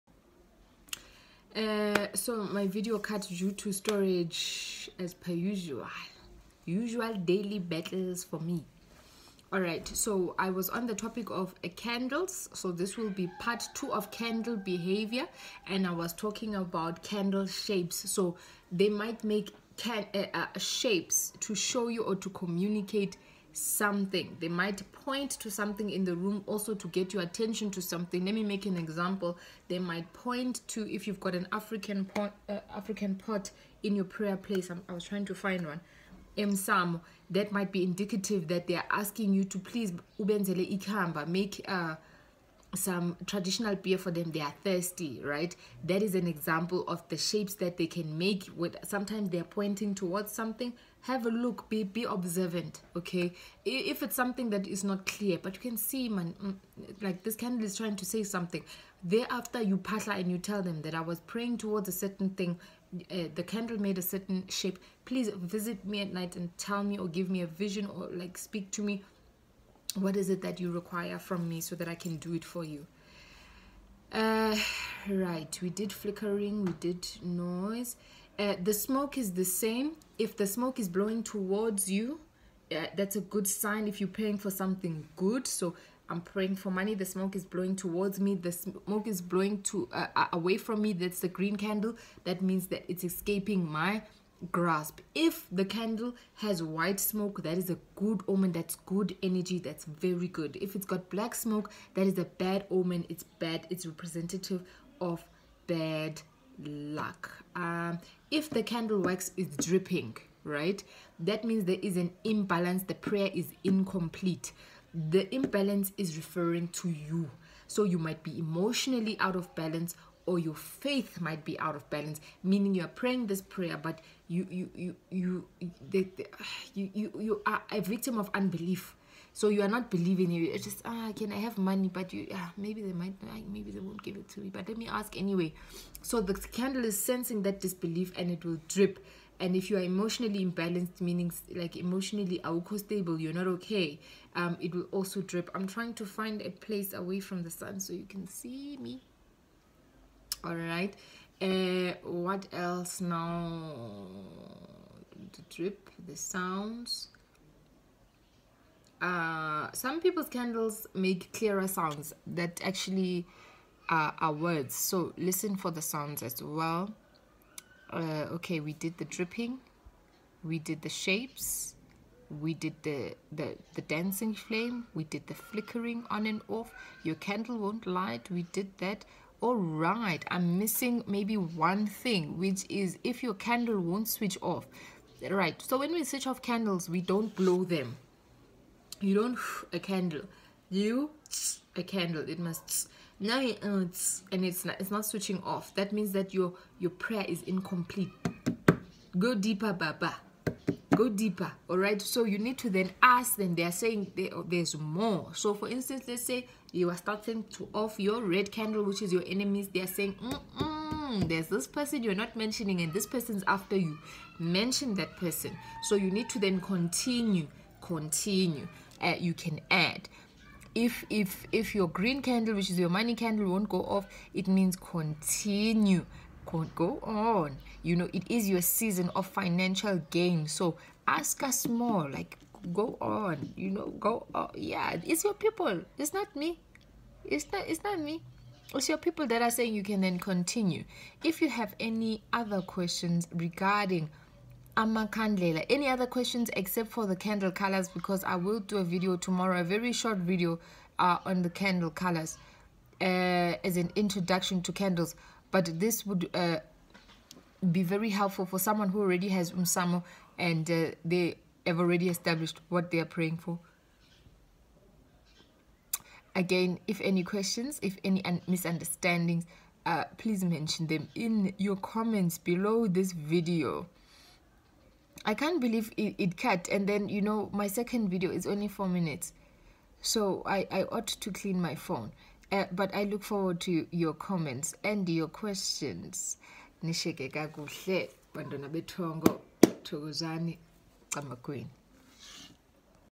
Candle Readings Part 2
Looking out for candle shapes, wax and what to do when it won’t go off.